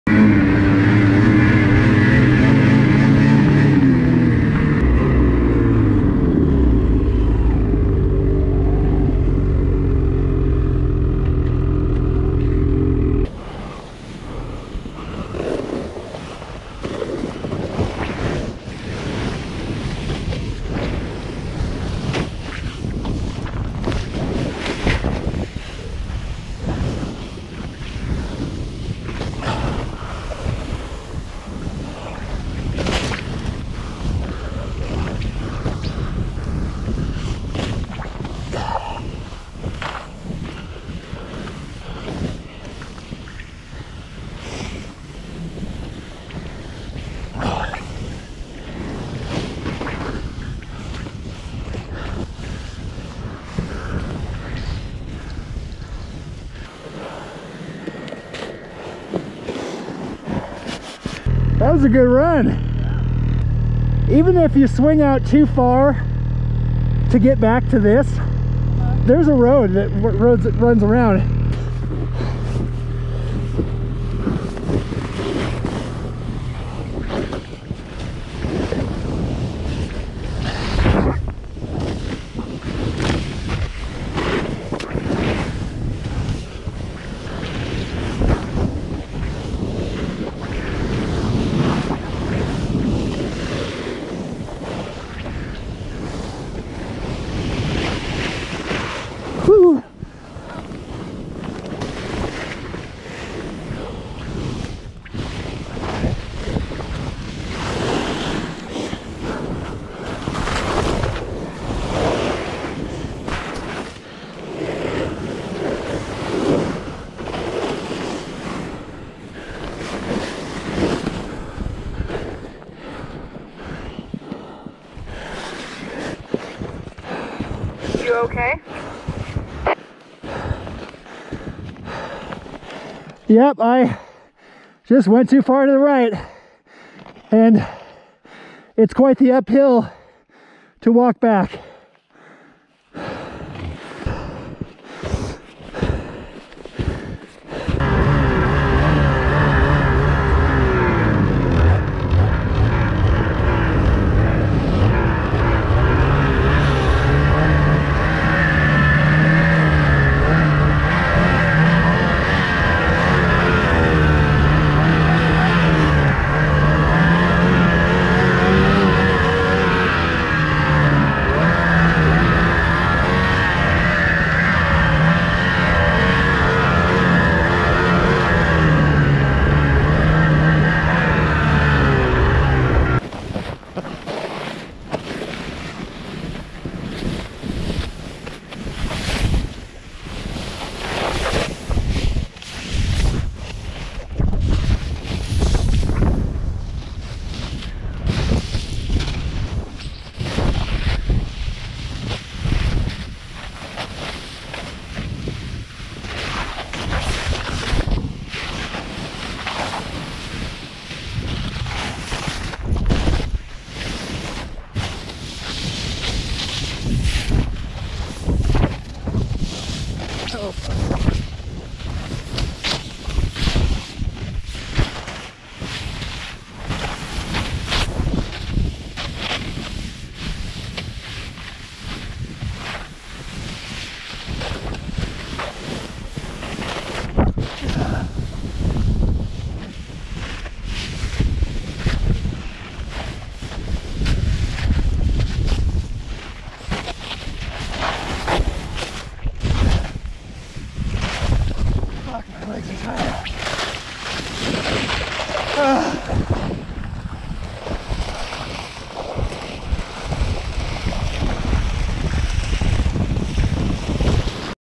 Sled Access Snowboarding WA First Try November 21, 2020
Snoqualmie still isn't open, but we don't wait for permission to get the goods.

November 21, 2020 Washington State Cascades backcountry powder.

2015 Ski-Doo Summit SP 600 E-Tec 154 x 2.5"
2019 Ski-Doo Summit SP 854 E-Tec 154 x 3"
2014 K2 Ultra Dream 164